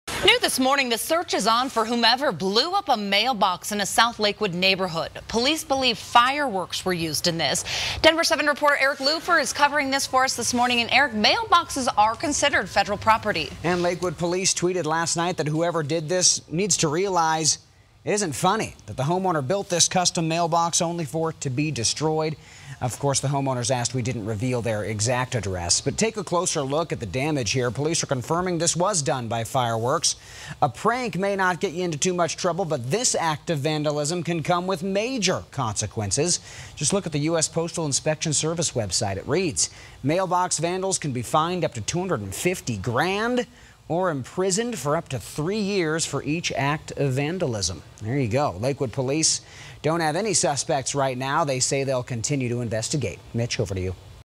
Police searching for suspect or suspects who blew up mailbox in Lakewood
The Lakewood Police Department is warning the vandal or vandals who blew up a mailbox that they committed a serious crime.